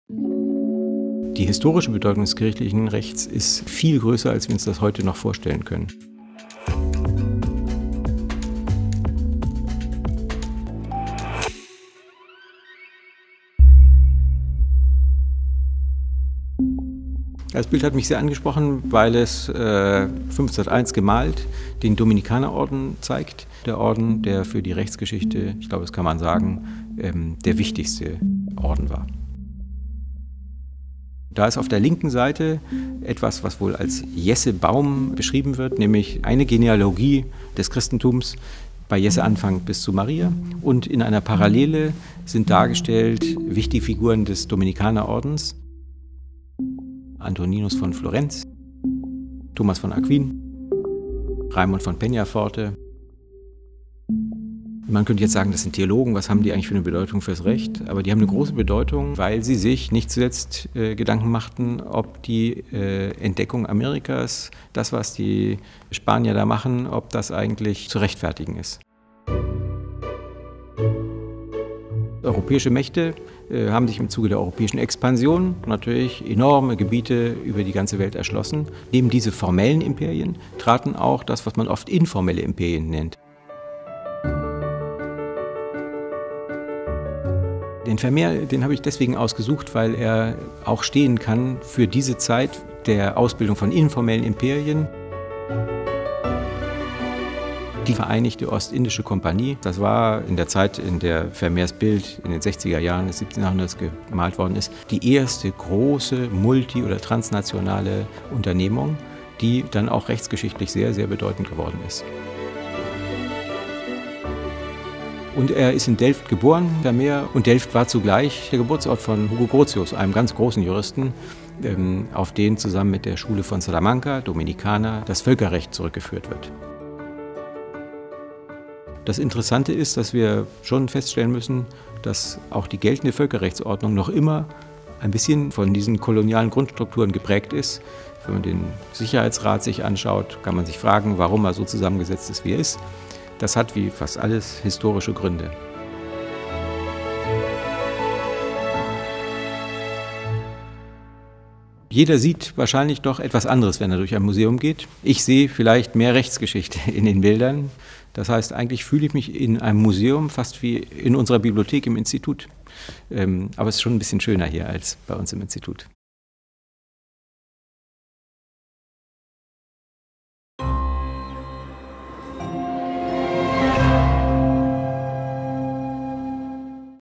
Gastkommentar: Kirchliche Macht und weltliches Recht mit Rechtshistoriker Thomas Duve
Was sieht ein Rechtshistoriker in den Werken der Städel Sammlung? In diesem Gastkommentar eröffnet Thomas Duve (Direktor am Max-Planck-Institut für europäische Rechtsgeschichte) seine individuelle Sichtweise auf die Kunstwerke im Städel Museum. Er schaut sich den "Stammbaum der Dominikaner" von Hans Holbein d. Ä. an und führt aus, warum der Dominikanerorden von besonderer Bedeutung für die Rechtsgeschichte war. Am Beispiel von Johannes Vermeers "Der Geograf" blickt er auf das Delft des 17. Jahrhunderts und die Ausbildung von informellen Imperien.

Die Werke in unserer Digitalen Sammlung
Hans Holbein d. Ä., Stammbaum der Dominikaner (1501):